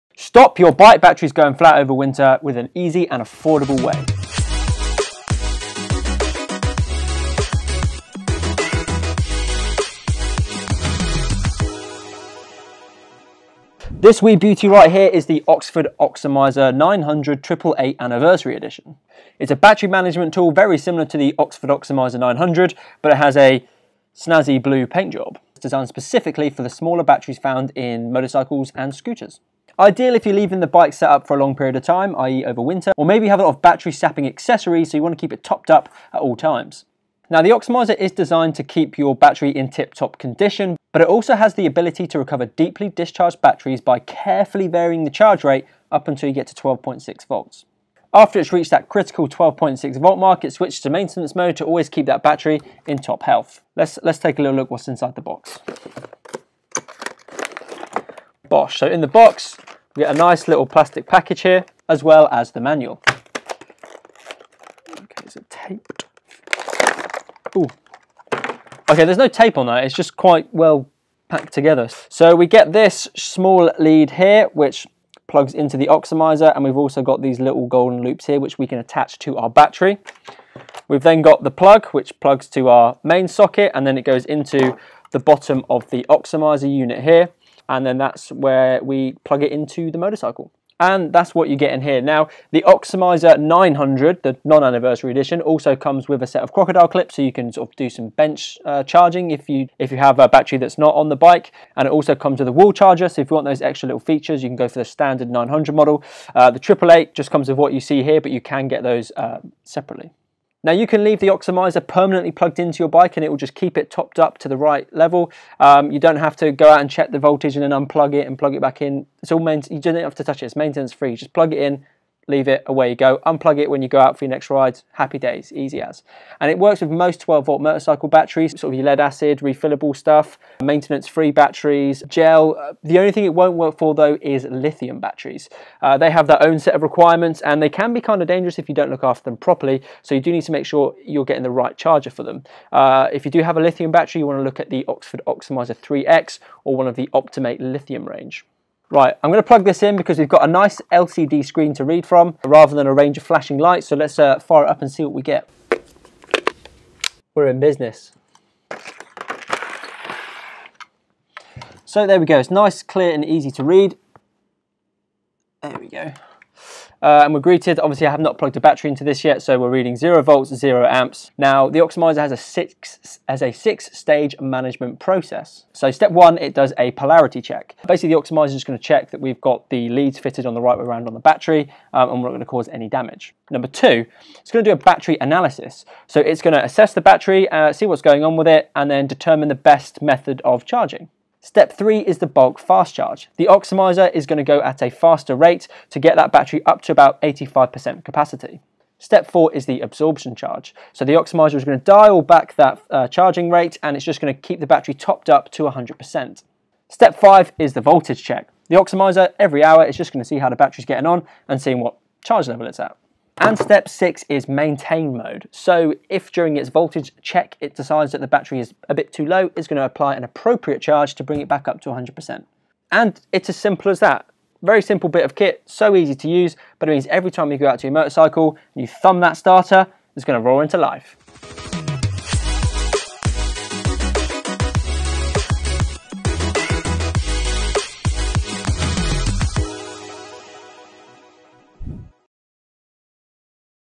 NO More DEAD Batteries | Oxford Oximiser 888 Review
HOW TO KEEP YOUR MOTORCYCLE BATTERY IN TOP CONDITION.

In Tom's hands today, he has the (deep breath) Oxford Oximiser 900/888 Charger Anniversary Edition... phew that's a long name for a small motorcycle battery charger.

Cursed with a falling battery level through winter whilst the bike is stored in hibernation?
Or have a battery draining alarm system?
A maintenance charger like the Oxford Oximiser will do the job at a cracking price to boot.

Intro - 0:00-00:12
Overview  - 00:13-00:54
In the Box - 00:55-02:04
Battery Types - 02:05-02:27
Plugged In & Screen - 02:28-02:50
Stages - 02:51-03:46
Thumb it - 3:46-4:04